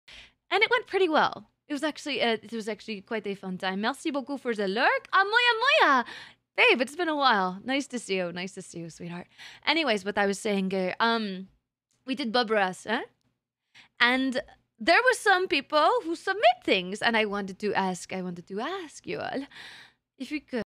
Cotton catches me lurking
This is a clip from CottontailVA's Twitch stream entitled MOMMY'S BACK, have you behaved BIG DROP TOMORROW!!!

This is a fan-made and uploaded video. CottonTailVA retains all rights to the stream content.